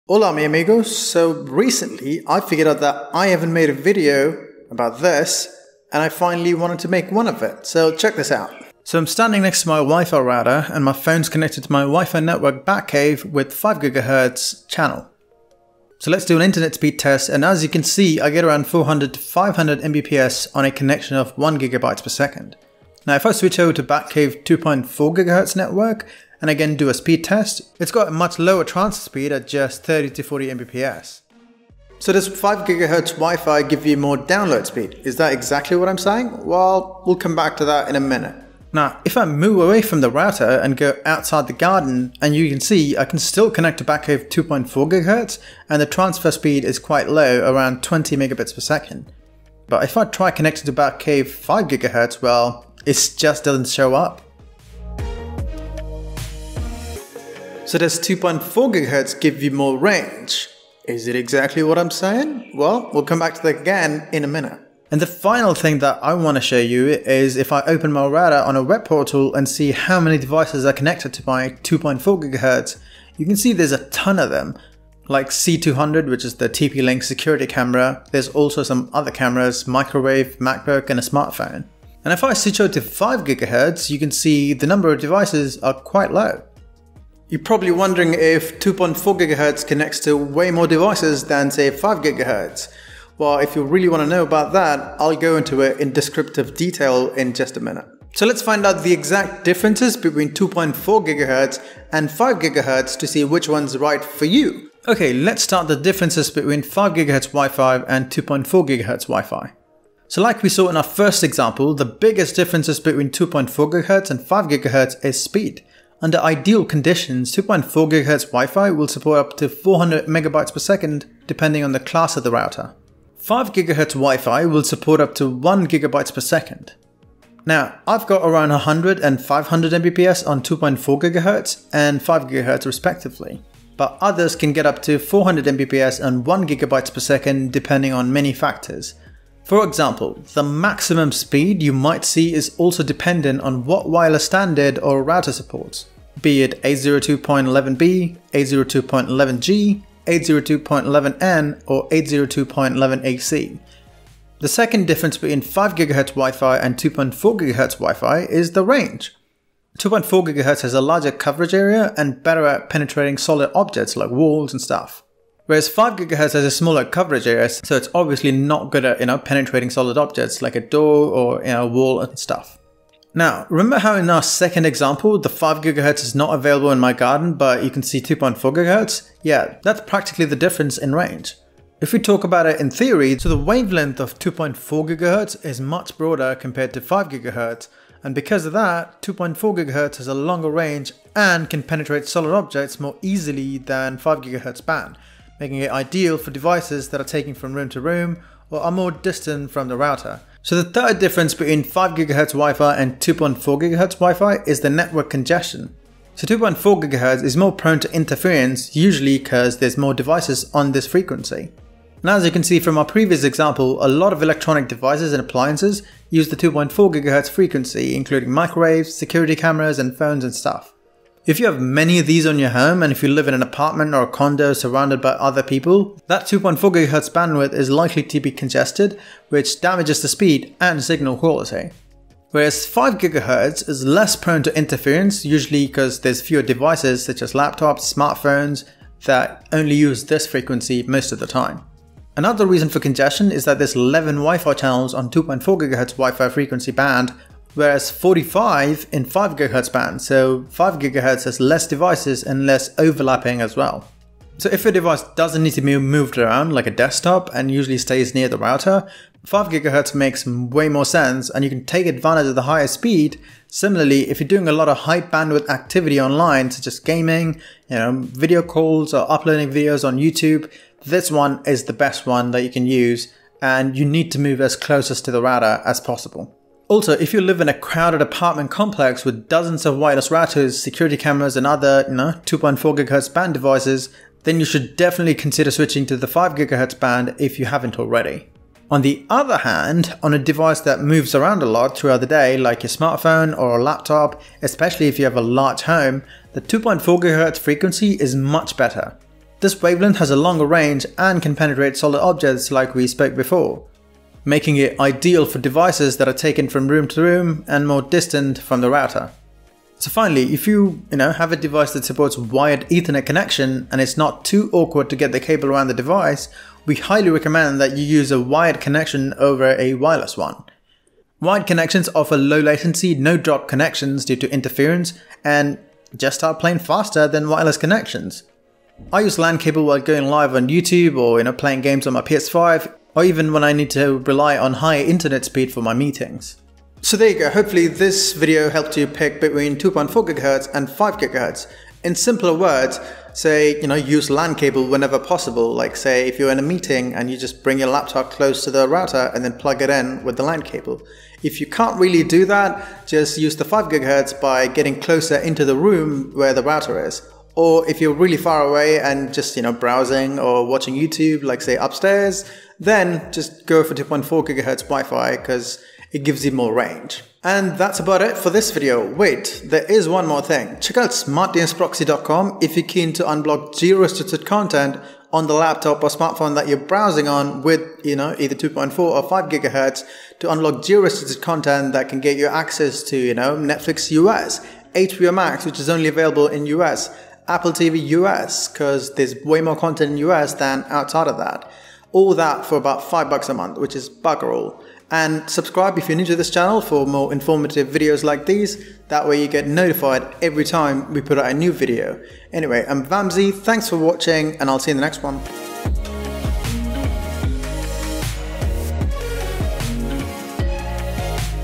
2.4GHz vs. 5GHz WiFi: What's the difference and how to use it?
Let’s find out what exactly is the diff between the 5GHz wifi vs 2.4GHz wifi network. Which is right for you when and how to change frequency settings on your router.

Useful links